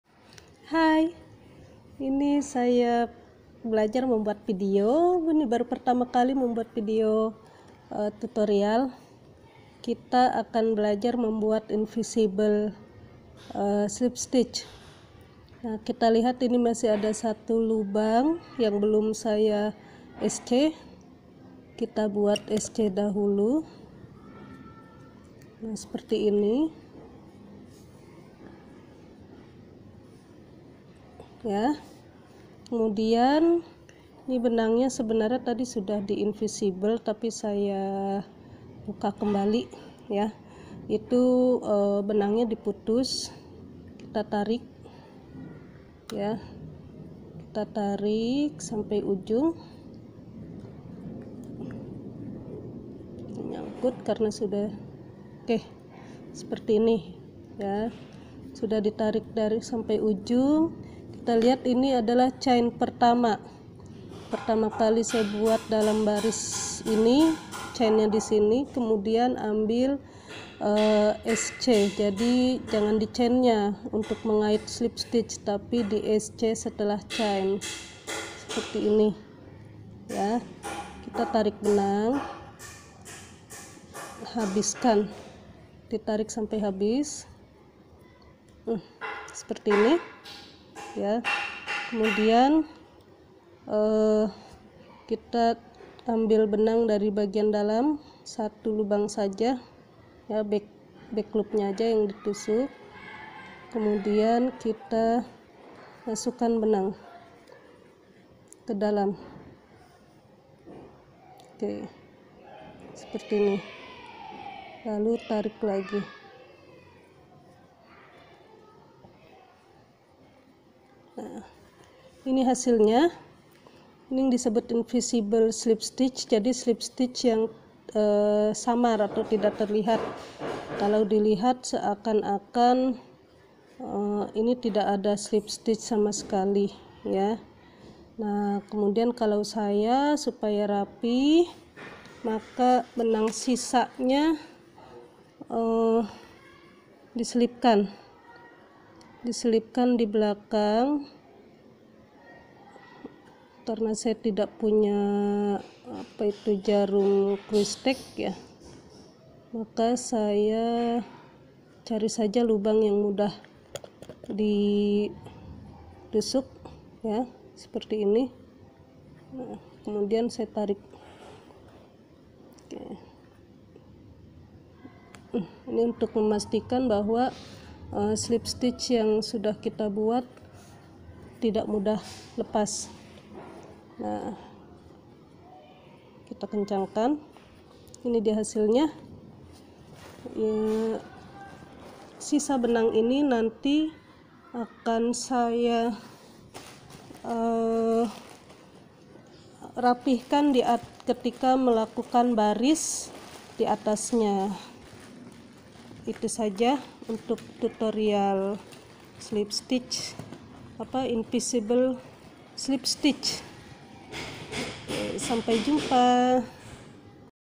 Invisible slipstich (tusuk mati tersamarkan)
Tutorial Membuat tusuk mati Samar dalamnya merajut crochet.